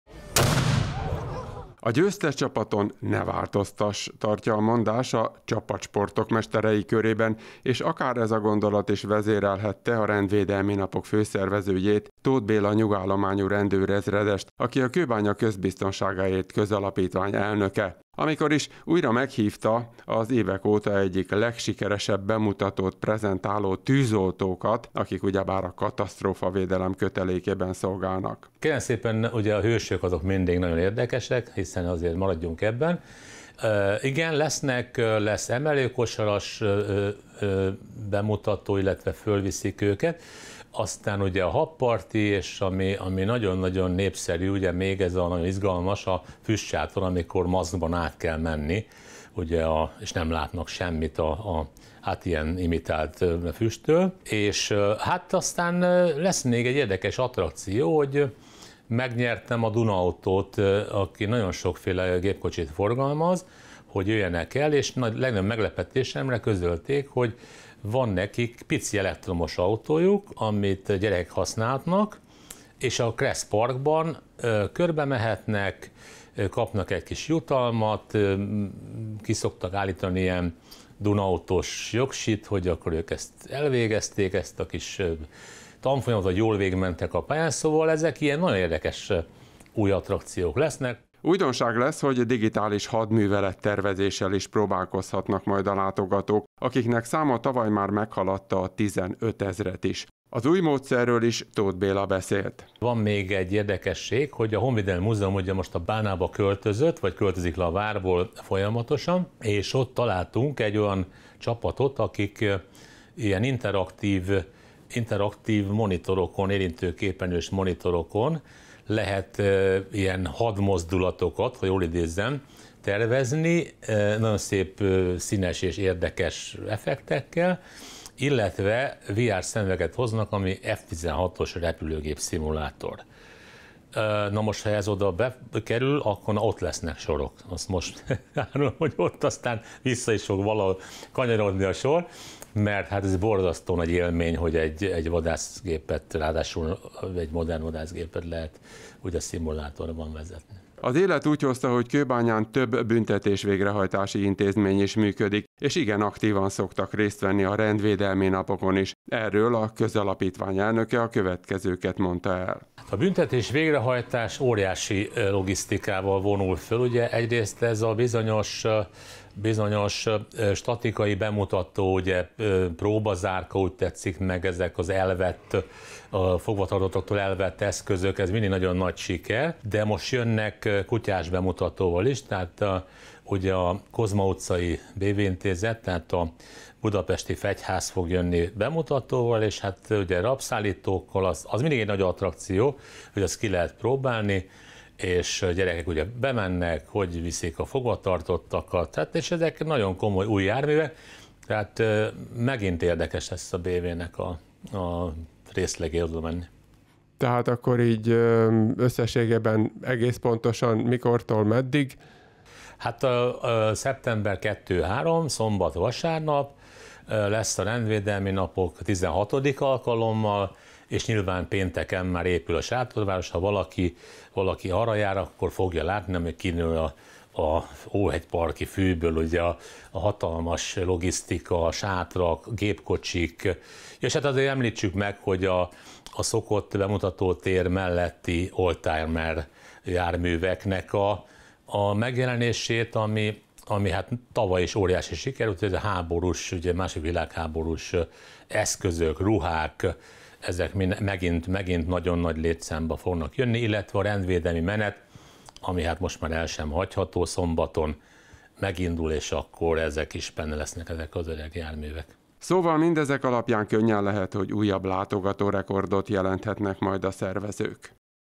TV10 | Rendvédelmi Napok 2023 | Előzetes
Az idei, immár XVI. Kőbányai Rendvédelmi Napok időzítésével bölcs dolog volt megvárni azt, amíg a budapesti atlétikai világbajnokságon elhangzik az utolsó startjel. A rendvédelmi dolgozók közül ugyanis igen sokan ott teljesítenek szolgálatot, felügyelik a versenyzők és a közönség biztonságát.

Így aztán szeptember első hétvégéjén kerül sor a hagyományos seregszemlére az Óhegy parkban. Úgy hírlik, hogy sok egyéb mellett F-16-os vadászgép-szimulátorral is találkozhatnak majd a kilátogatók.

A közelgő Rendvédelmi Napokról az esemény főszervezőjét, Tóth Béla ny. rendőr ezredest, a Kőbánya Közbiztonságáért Közalapítvány elnökét kérdeztük.

Szerkesztő-riporter: Martin-Kovács Miklós, operatőr-vágó: Szőke János